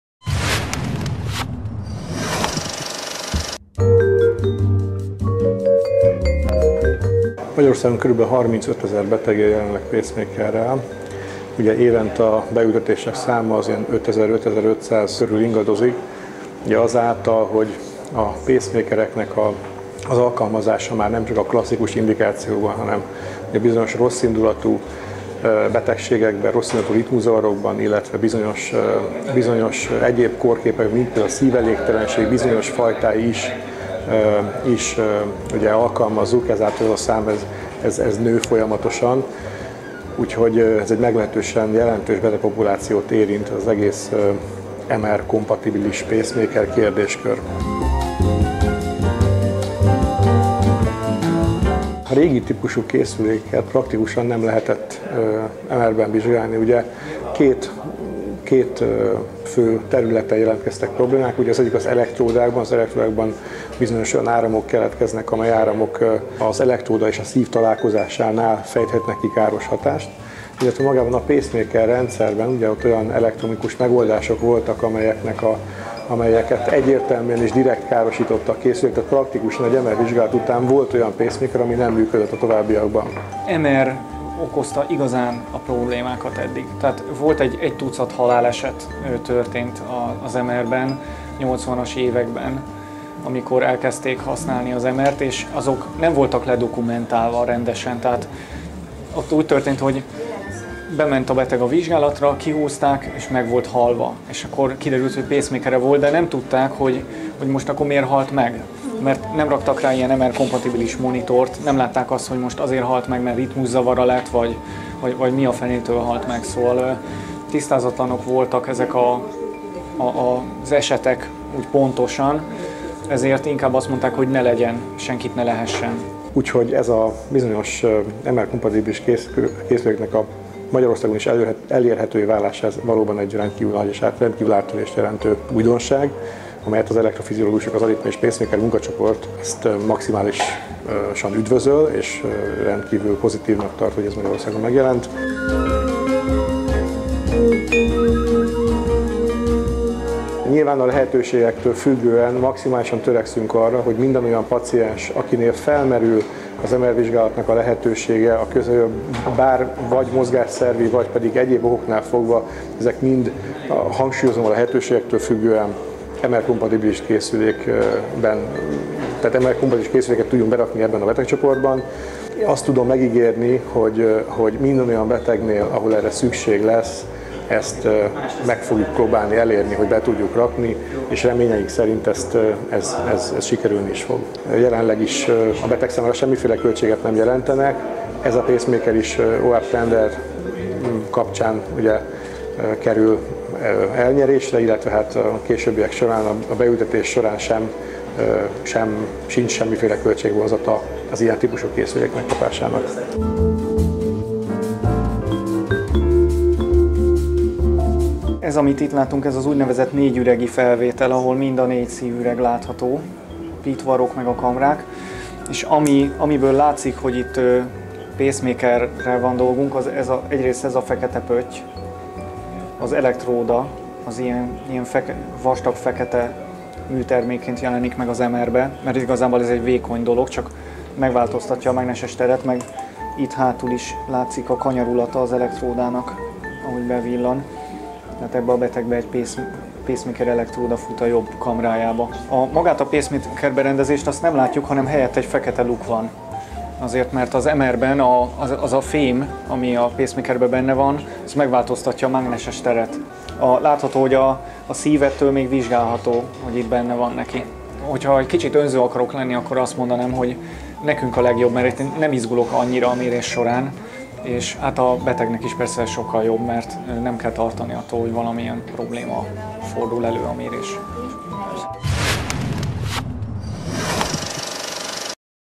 BetegszobaTV - MRI-kompatibilis Pacemaker
Végre Magyarországon is elérhető az MRI-kompatibilis pacemaker. E pár percben erről tudhatnak meg többet.